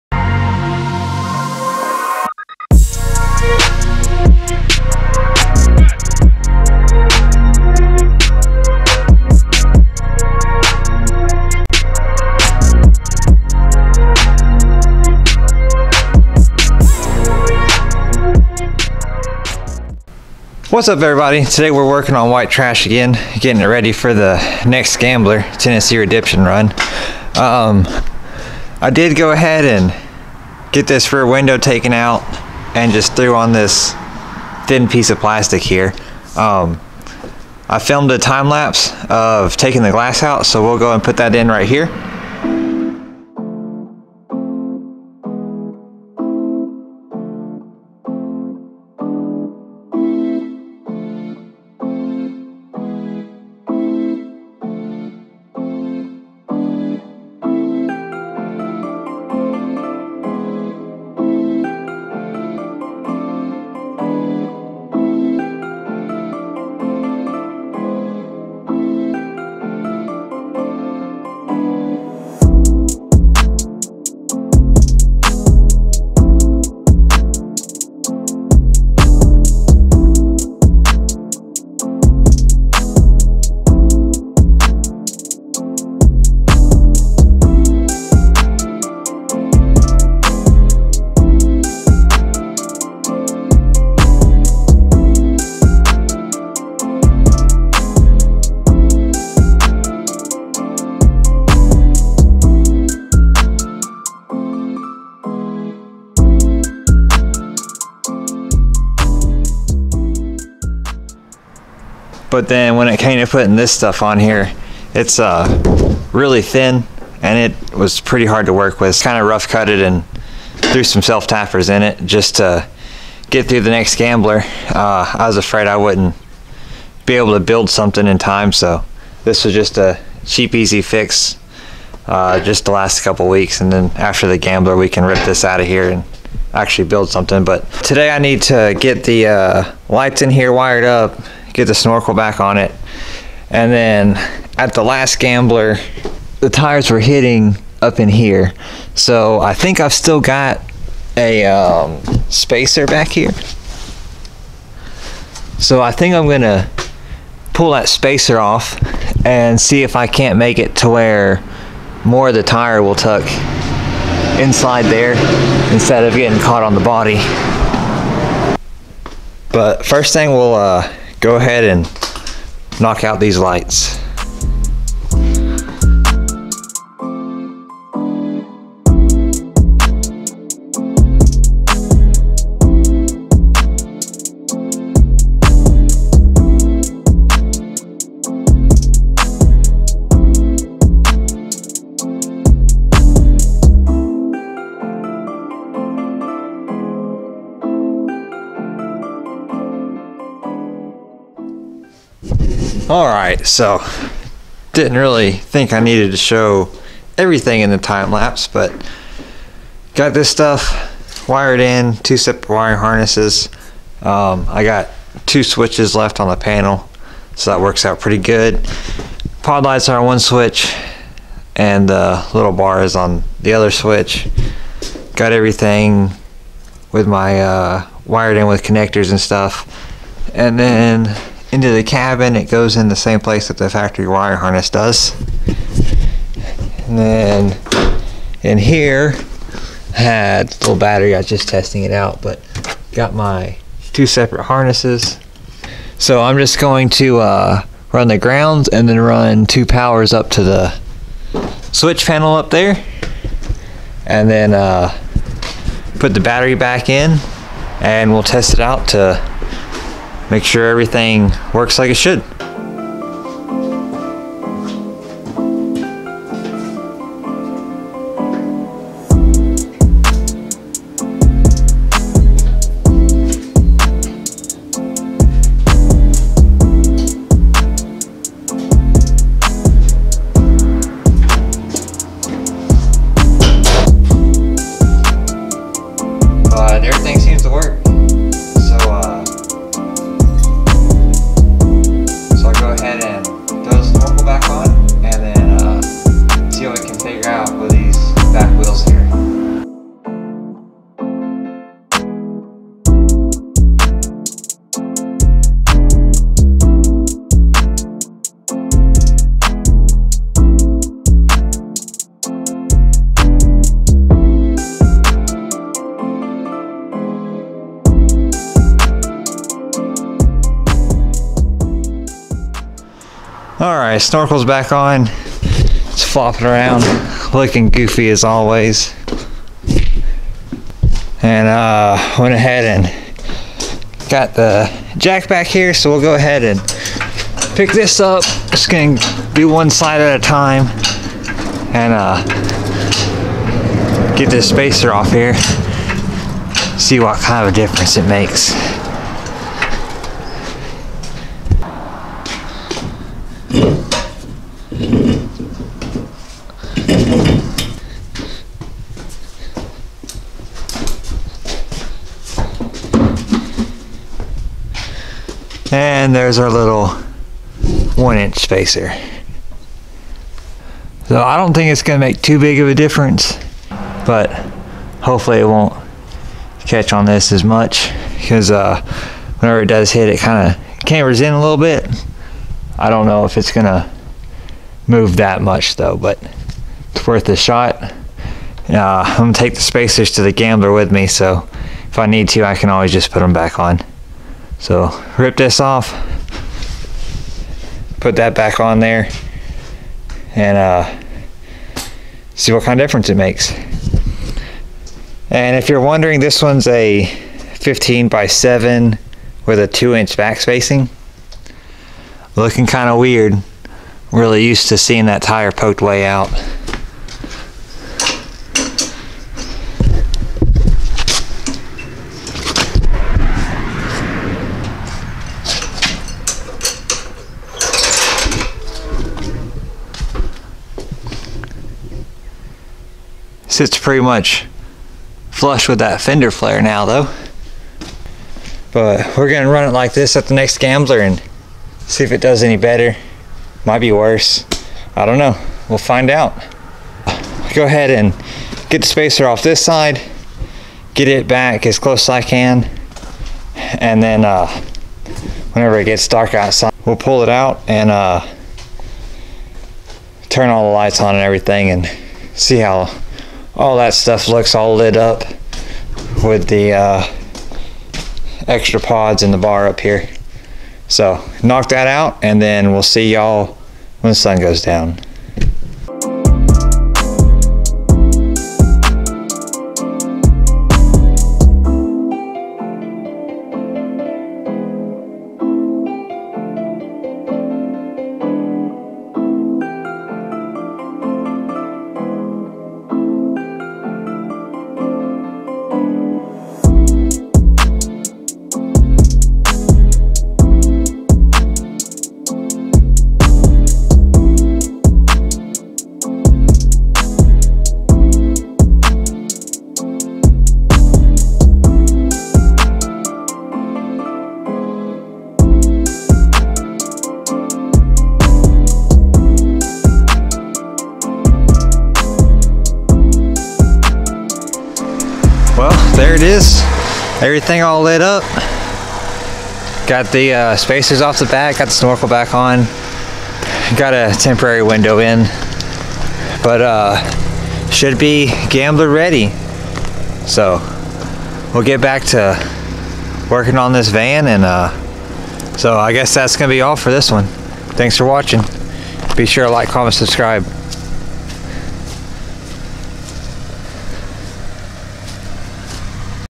LIFTED CIVIC GAMBLER 500 REDEMPTION RUN 2020 PREP!!
Getting White Trash (lifted civic) ready for the next gambler 500 event! Wiring lights, putting snorkel back on, ect